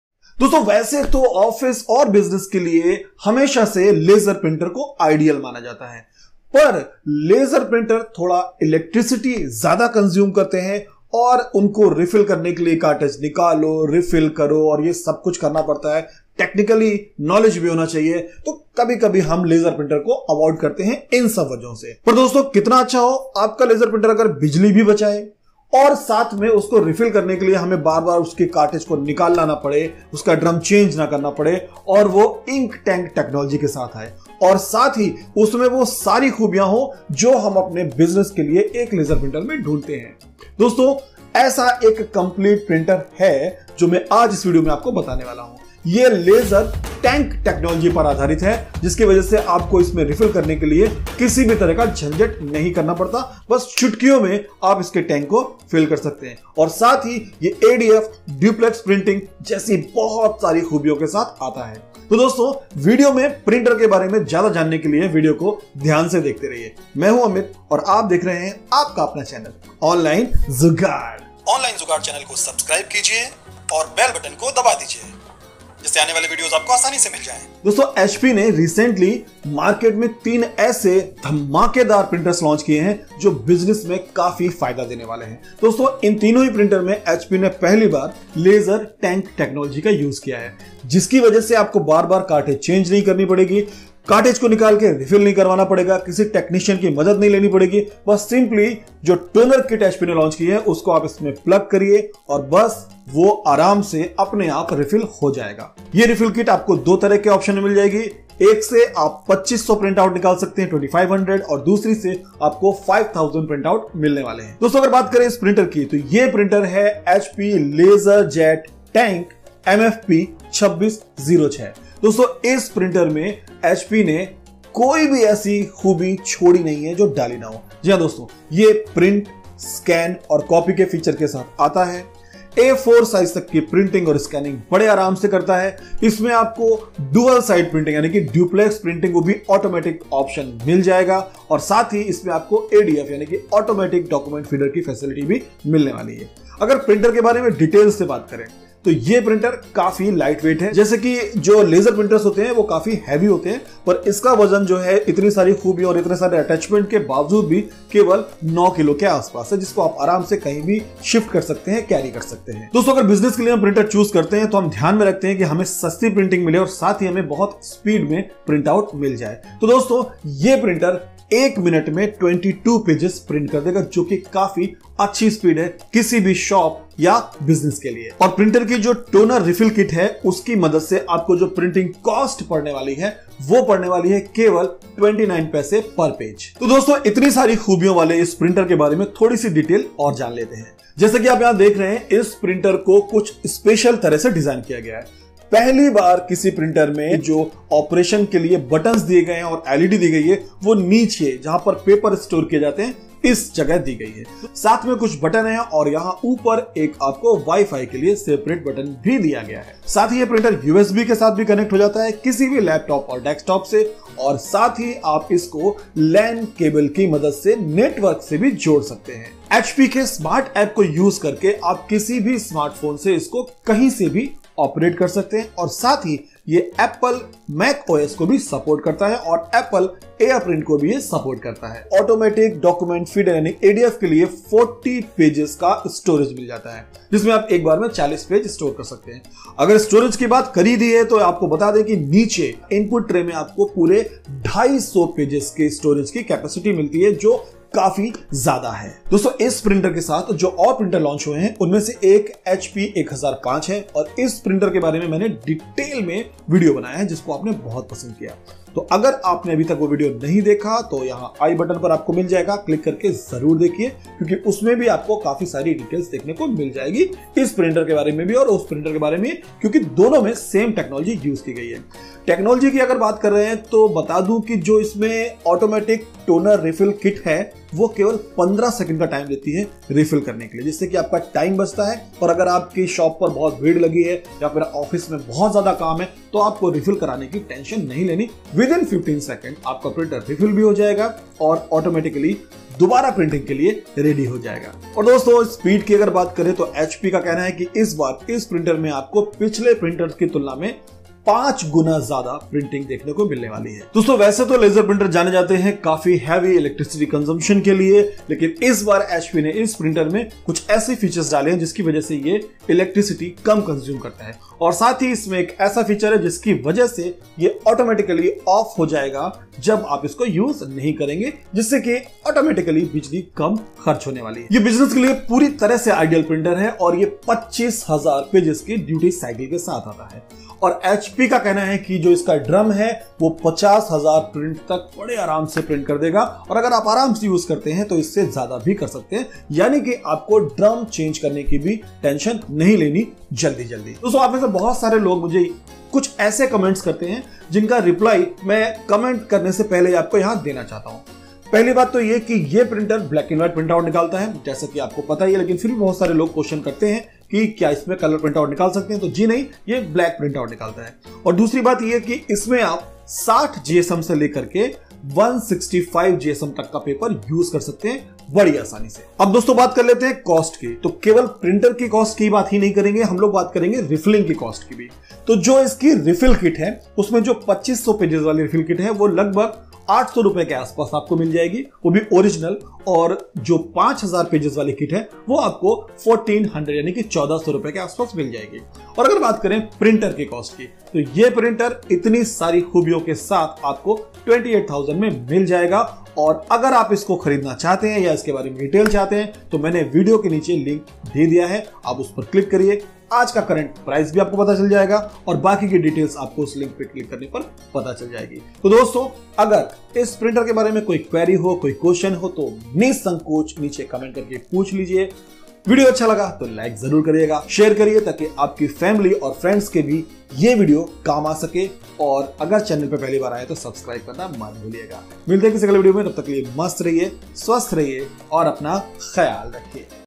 Best Duplex Laser Printer for Business || Best Printer for Photocopy shop
Namskar Dosto,
iss Video me apke liye hai ek behetreen printer jo aap apne business / office ke liye use kar sakte hain . ye ek laser printer toh hai but tank wala wo bhi ADF and auto duples ke saath
printer ke bare me jada details janne ke liye video dhyan se dekhiye